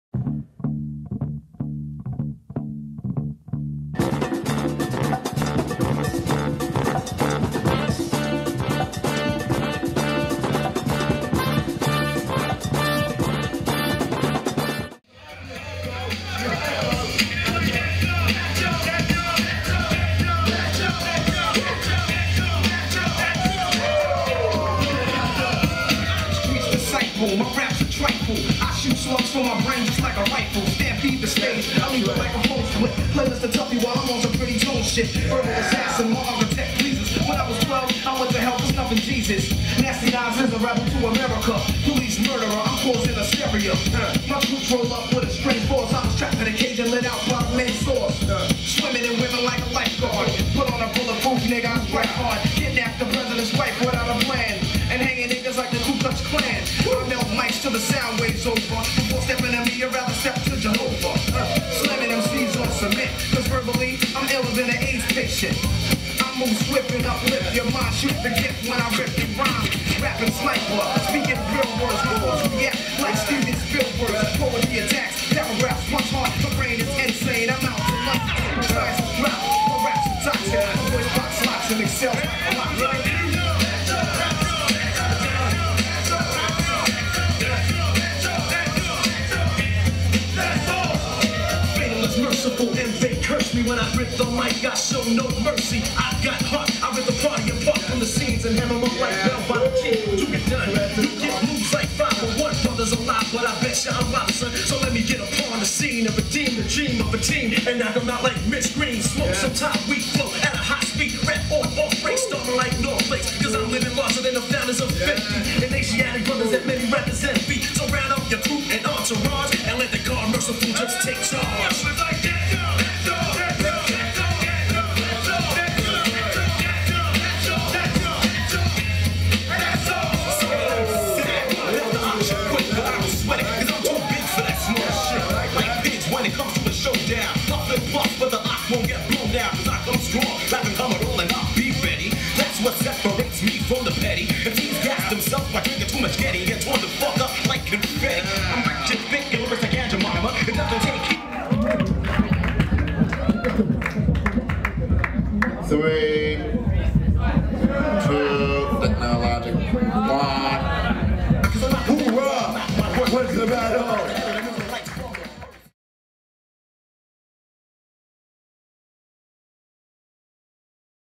Skillz-o-Meter 3 - Huitième de finale - Whorah vs Onton
Huitième de finale de l'événement « Skillz-o-Meter 3 » entre Whorah (Beast Coast, New York / NY / USA) et Onton (Supernaturalz, Toronto / ON / Canada) qui a eu lieu le samedi 11 mai 2013 dans la ville de Montréal au Québec, Canada.